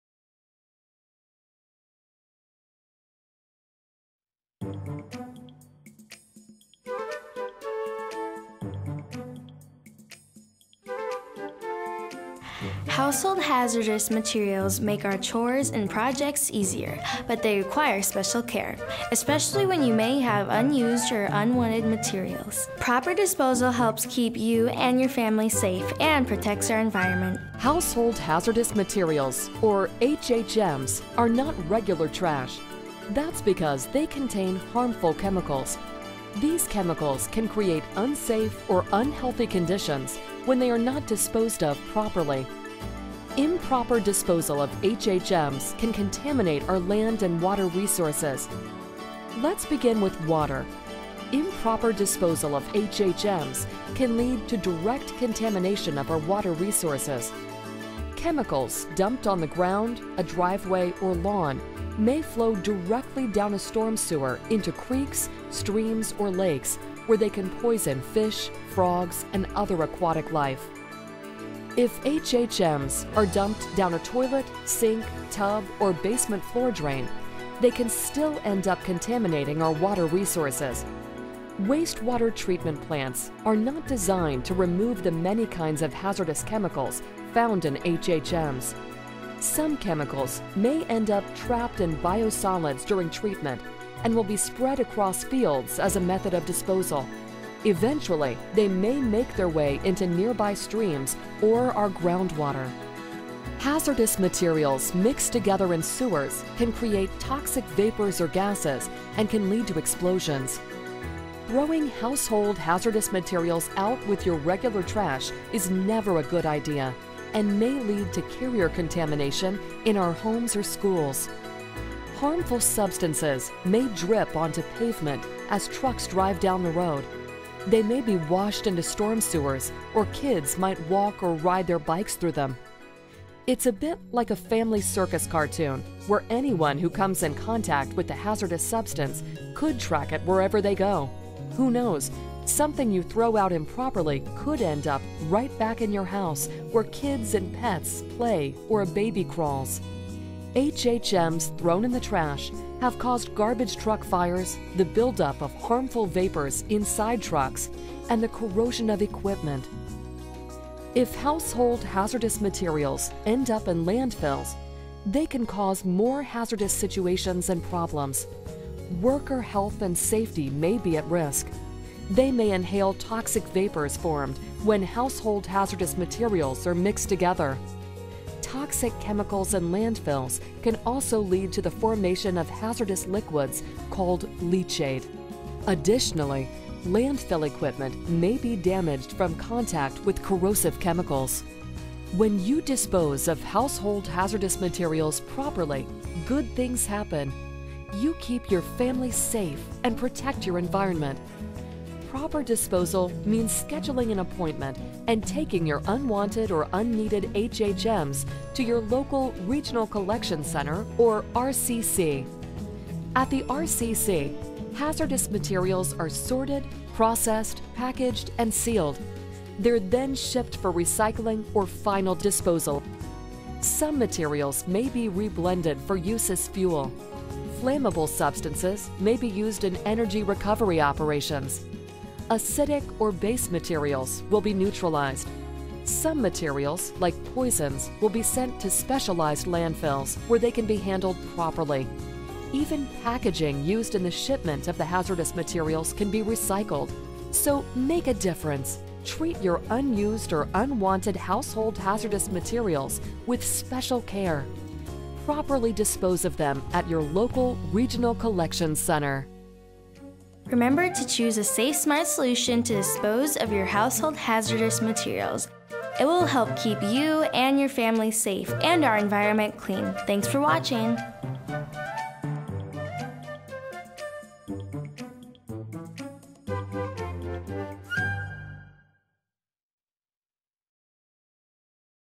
Why Dispose of Household Hazardous Materials (HHMS) Properly, Iowa DNR
Household hazardous materials make our chores and projects easier, but they require special care, especially when you may have unused or unwanted materials.  Proper disposal helps keep you and your family safe, and protects our environment.

Household hazardous materials, or HHMs, are not regular trash. That's because they contain harmful chemicals. These chemicals can create unsafe or unhealthy conditions when they are not disposed of properly.  Improper disposal of HHMs can contaminate our land and water resources.

Improper disposal of HHMs can lead to direct contamination of our water resources.   Chemicals dumped on the ground, a driveway or lawn may flow directly down a storm sewer into creeks, streams or lakes where they can poison fish, frogs, and other aquatic life.

If HHM's are dumped down a toilet, sink, tub or basement floor drain, they can still end up contaminating our water resources. Wastewater treatment plants are not designed to remove the many kinds of hazardous chemicals found in HHMs.  Hazardous materials mixed together in sewers can create toxic vapors or gases, and can lead to explosions.

Throwing household hazardous materials out with your regular trash may also endanger the health and safety of collection workers.  HHMs thrown in the trash have caused garbage truck fires, the buildup of harmful vapors inside trucks, and the corrosion of equipment.  Worker health and safety may be at risk. They may inhale toxic vapors formed when household hazardous materials are mixed together.

When you dispose of household hazardous materials properly, good things happen; you keep your family safe and protect your environment.   Proper disposal means scheduling an appointment and taking your unwanted or unneeded HHMs to your local Regional Collection Center or RCC.  At the RCC, hazardous materials are sorted, processed, packaged and sealed. They're then shipped for recycling or final disposal.   Some materials will be reblended for use as fuel.  Flammable substances may be used in energy recovery operations. Acidic or base materials will be neutralized. Some materials, like poisons, will be sent to specialized landfills where they can be handled properly. Even packaging used in the shipment of the hazardous materials can be recycled.

So make a difference, treat your unused or unwanted household hazardous materials with special care. Properly dispose of them at your local regional collection center.

To learn more, visit SafeSmartSolutions.org. When we change our ways, we change our world.